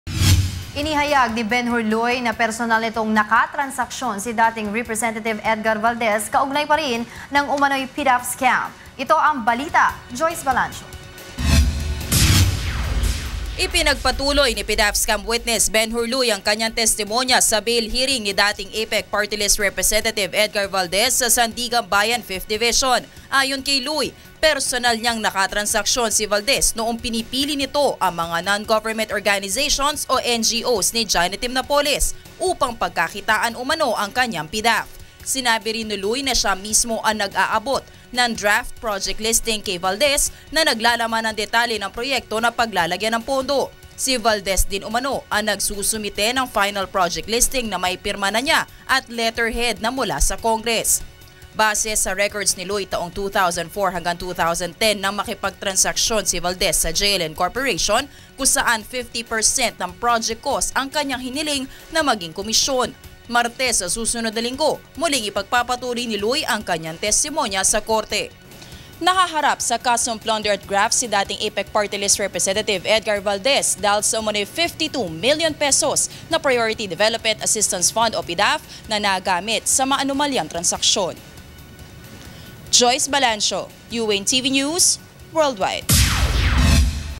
Dating Rep. Edgar Valdez, personal umanong naka-transaksyon ni Benhur Luy
AIRING DATE: August 26, 2015